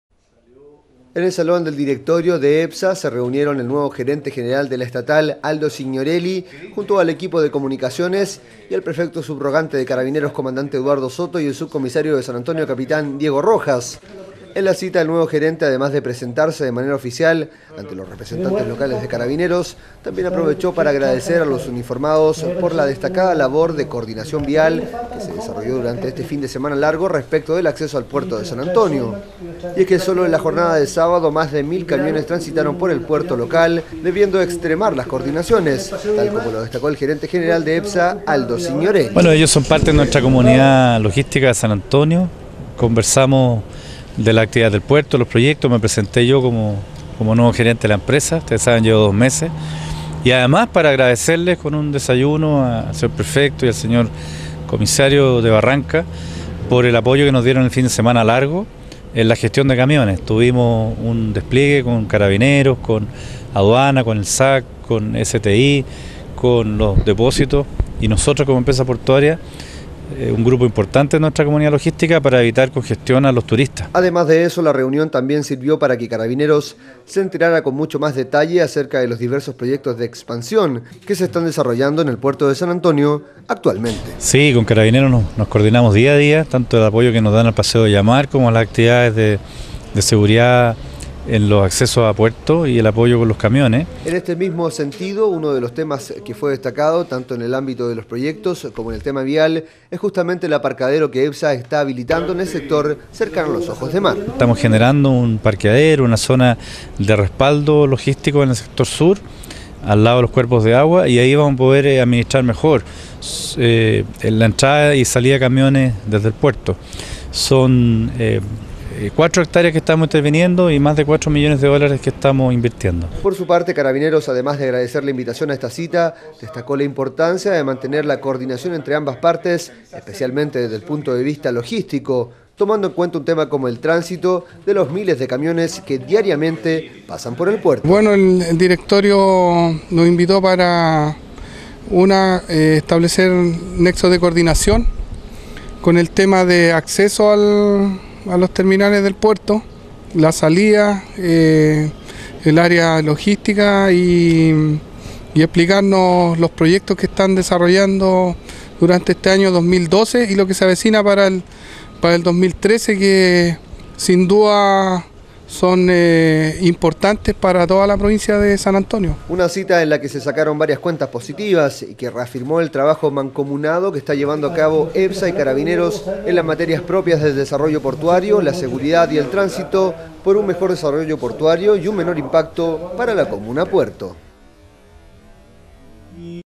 19 JUL 2012 REUNION_CARABINEROS_EPSA
UNA IMPORTANTE REUNIÓN DE COORDINACIÓN ENCABEZÓ LA GERENCIA DE LA EMPRESA PORTUARIA DE SAN ANTONIO Y CARABINEROS DE LA PREFECTURA LOCAL, EN EL MARCO DE LOS PROYECTOS DE EXPANSIÓN PORTUARIA Y DE LOGÍSTICA EN MATERIA DE TRANSITO. EN LA CITA, EPSA LE EXPUSO A CARABINEROS LOS DETALLES DE LOS PROYECTOS, ADEMÁS DEL TRABAJO VIAL QUE LLEVA A CABO PARA ORGANIZAR EL TRANSITO DE CAMIONES.